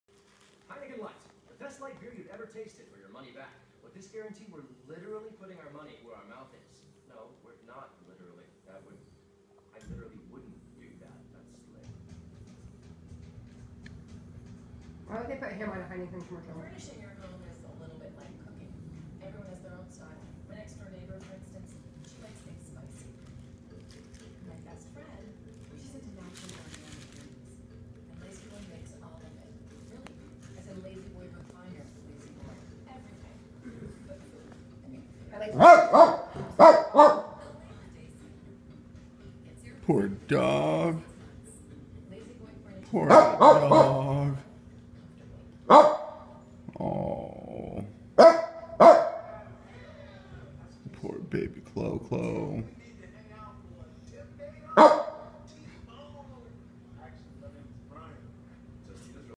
Chloe waiting for a nibble
Chloe wanted a piece of Kari's steak and egg burrito!